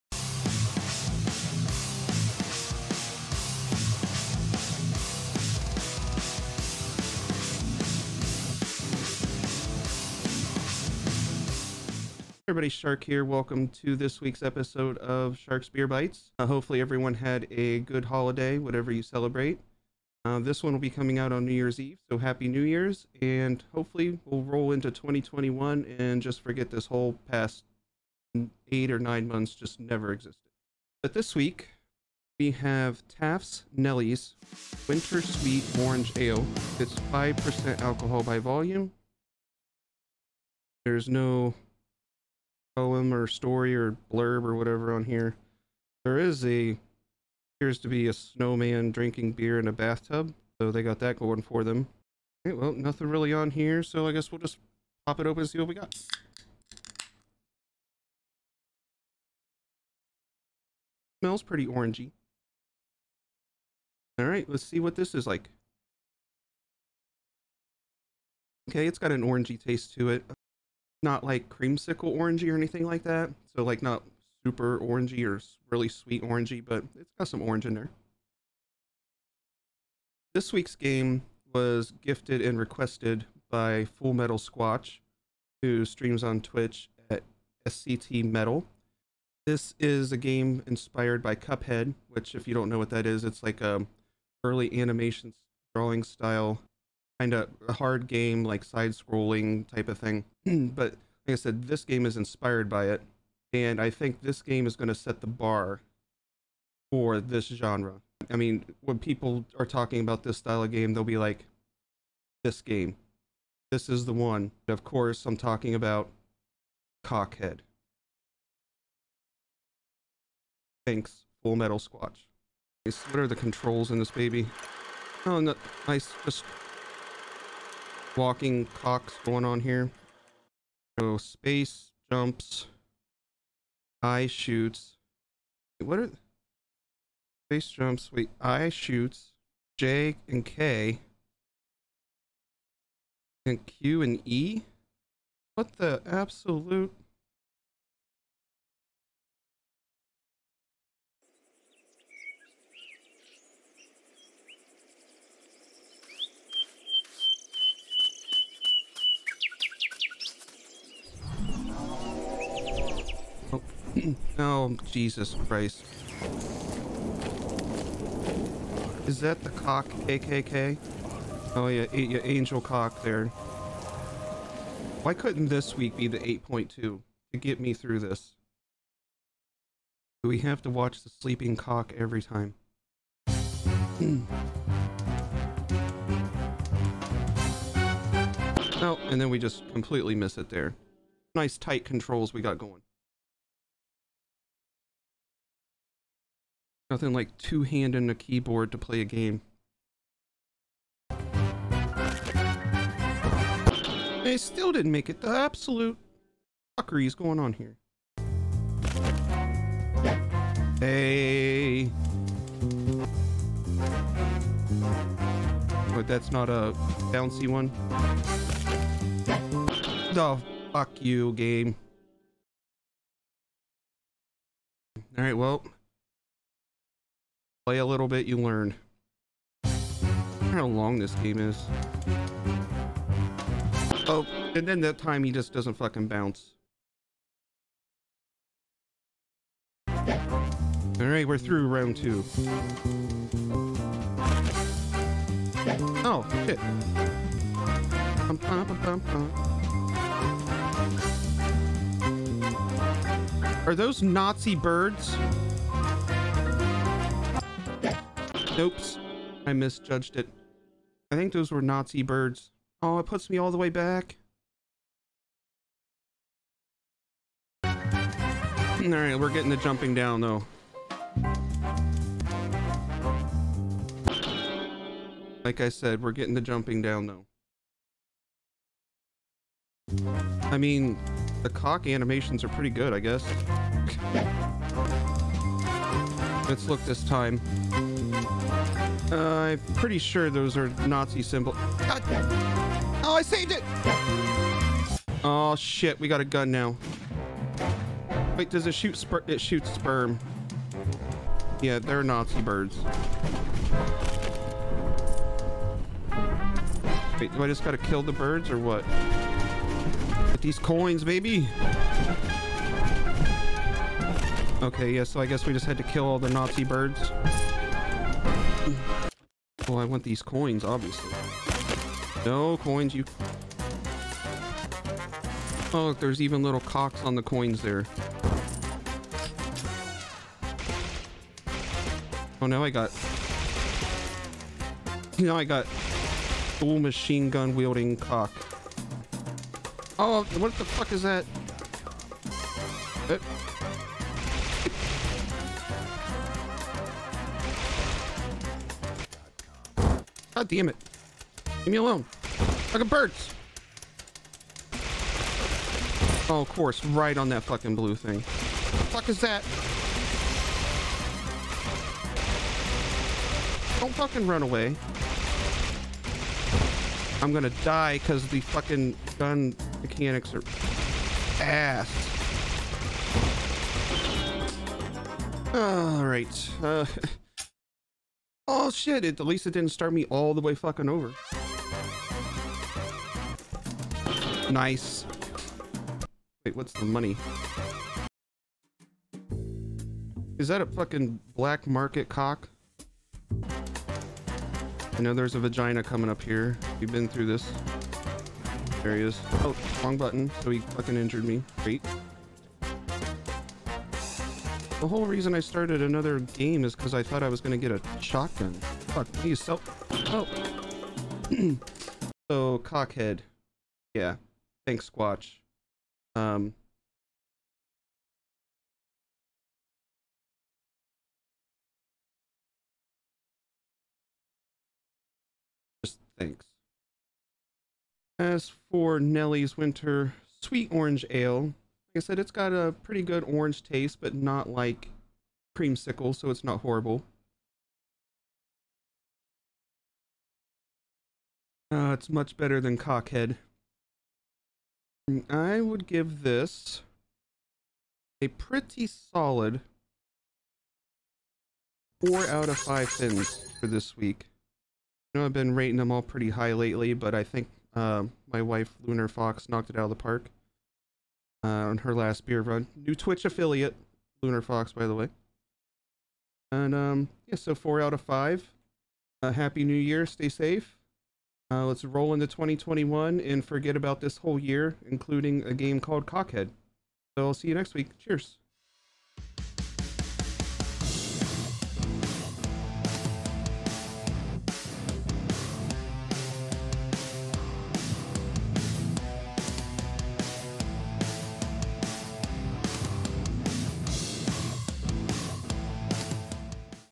BeerBytes Ep22: Taft's Nellie's Winter Sweet Orange Ale
This week we review a beer from Taft's Nellie's Winter Sweet Orange Ale

If you have any suggestions of beers to try, or have had this beer leave some comments below!